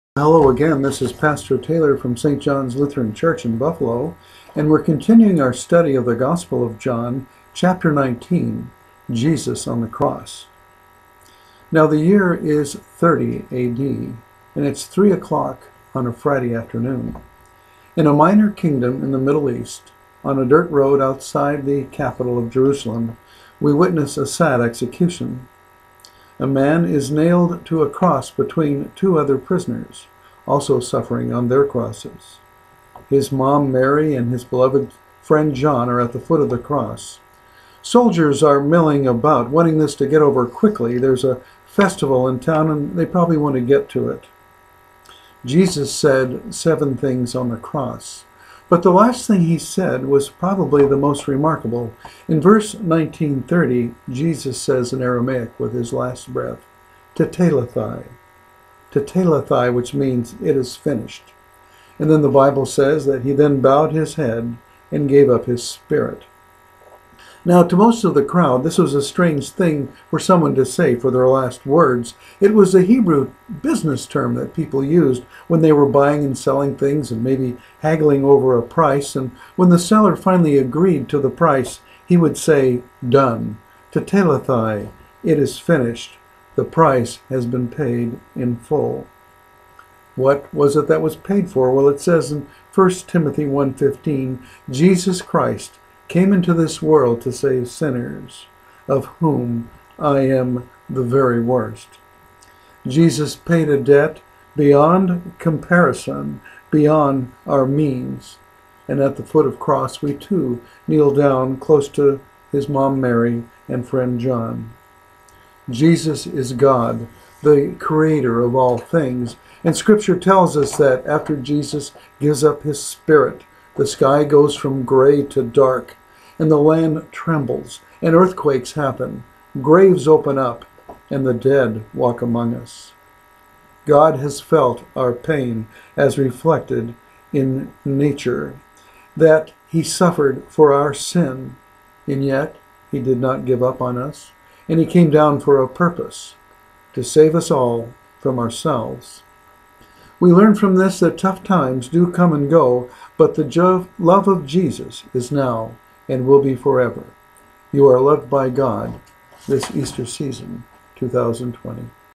2020 April 10 - Pastor Taylor Daily Devotional
Pastor Taylor expounds on John 19